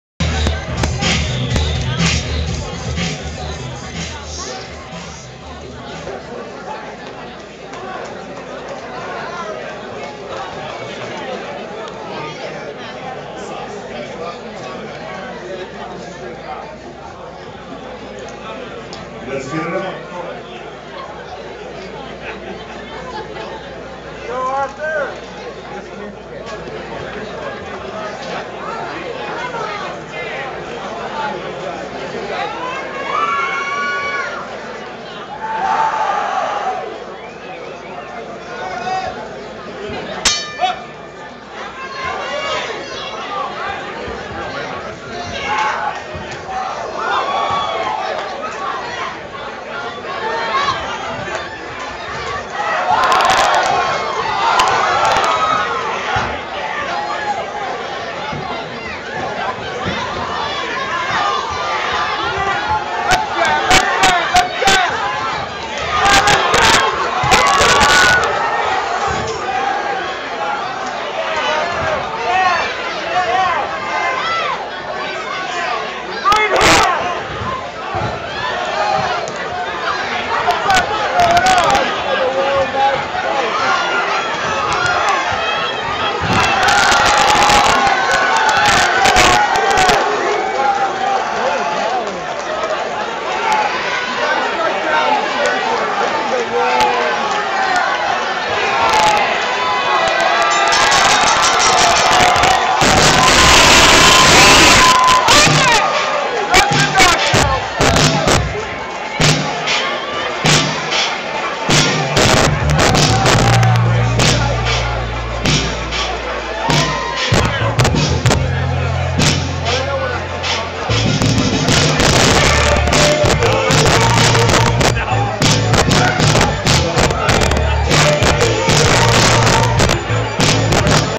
Arthur Bridges' Pro Boxing Debut - 05/29/09
Arthur Bridges' Professional Boxing Debut in Atlanta, Georgia on May 29, 2009.